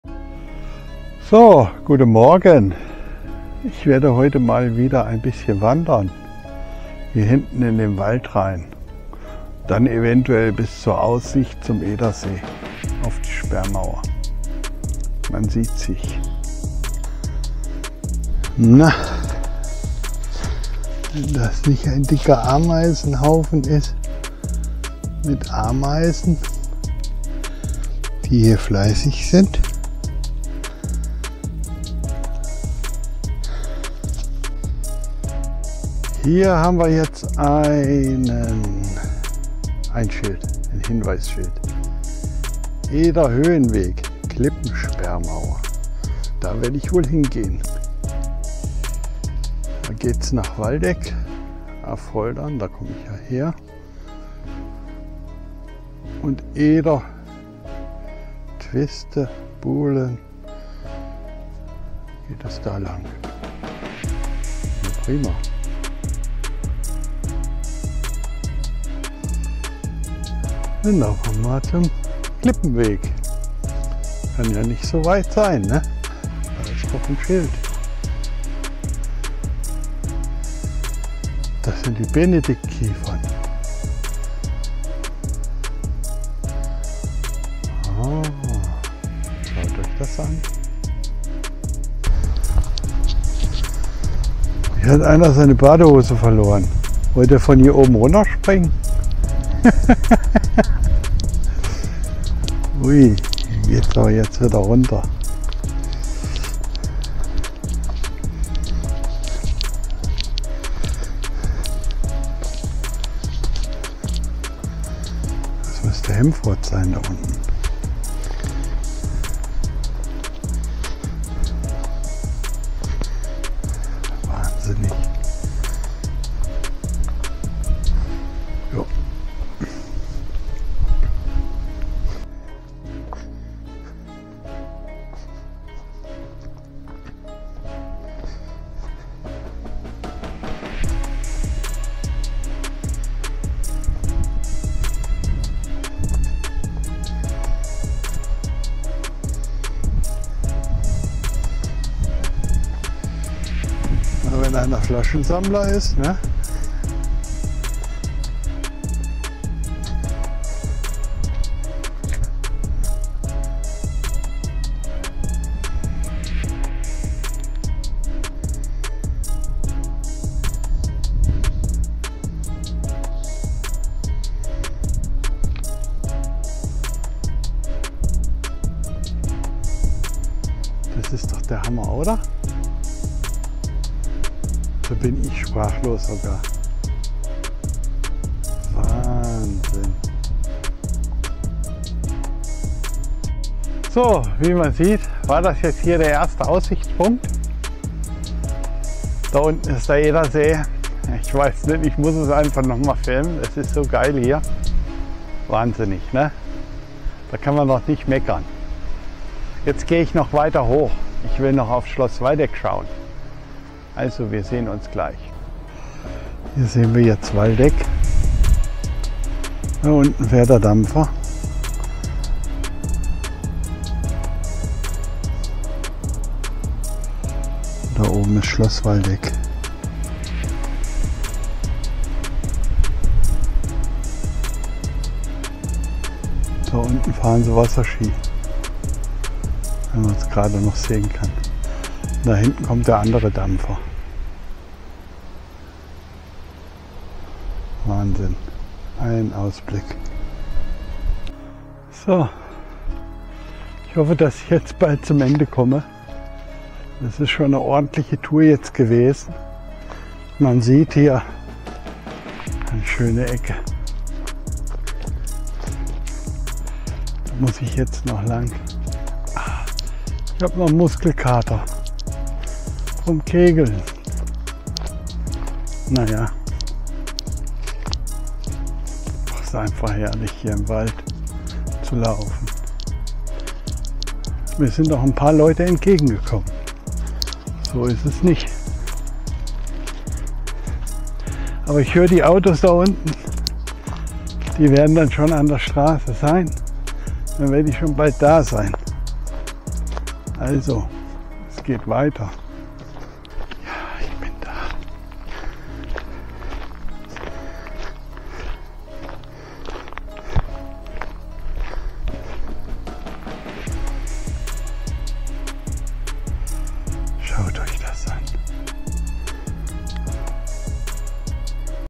Wandern mit Klaus am Edersee
Wenn Sie gerne wandern und die Natur lieben, ist der Edersee in Hessen, Deutschland, der perfekte Ort für eine Wanderung mit Ihrem Freund Klaus. Der Edersee ist einer der größten Stauseen Deutschlands und bietet eine atemberaubende Landschaft mit dichten Wäldern, malerischen Ufern und spektakulären Ausblicken.